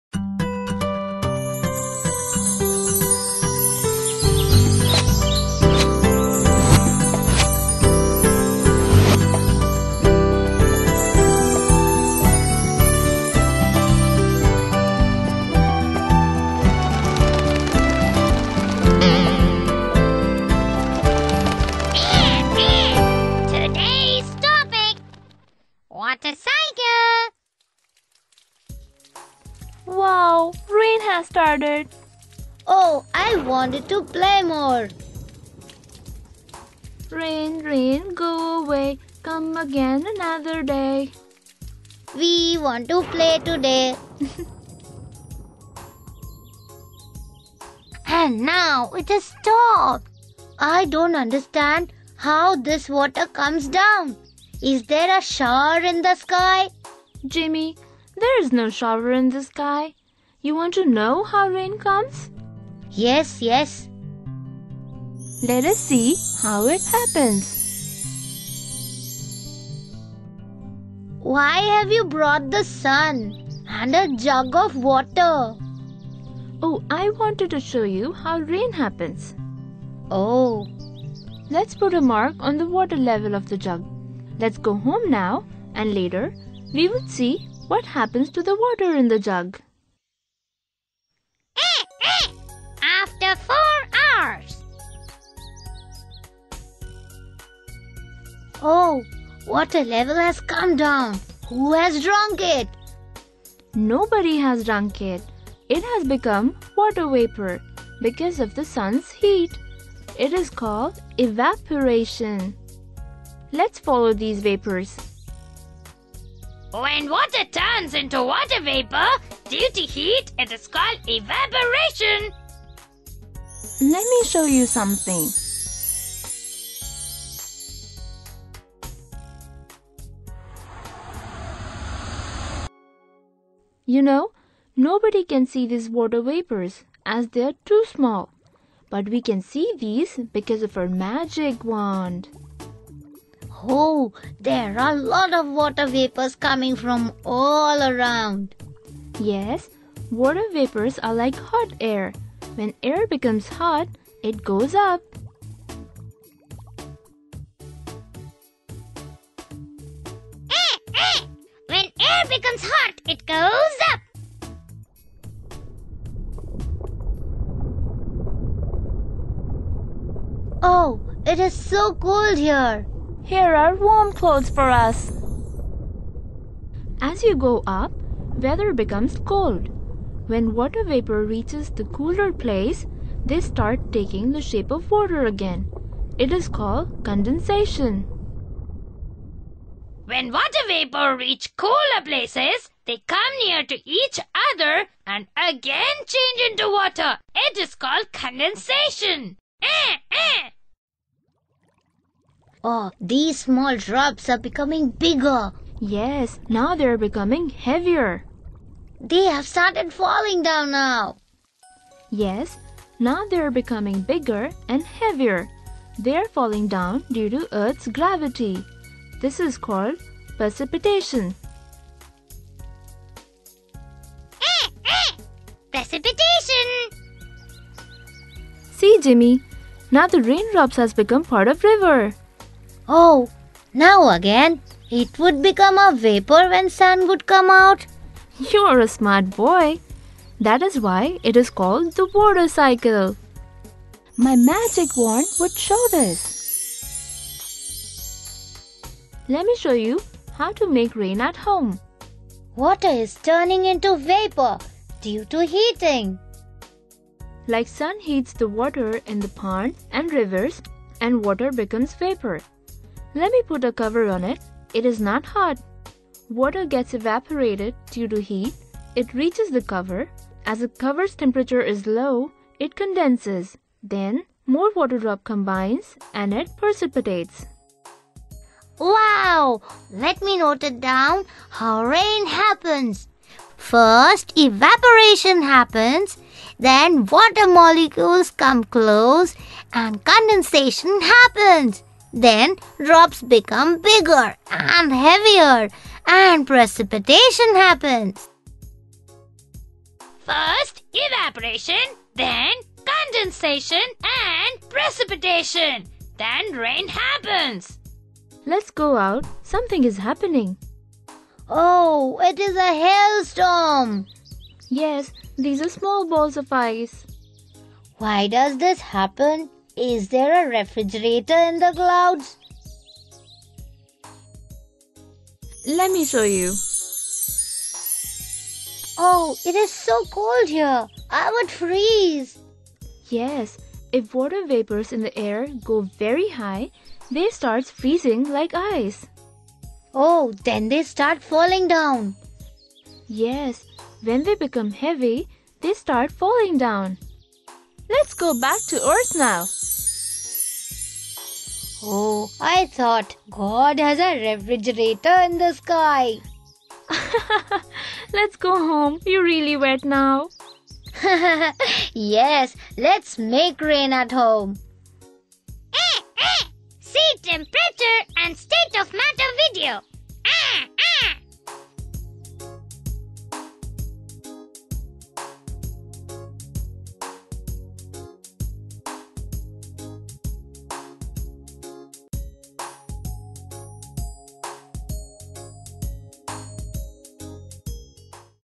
Water Cycle For Kids
A simple graphical video designed to convey the idea of Water Cycle to kids.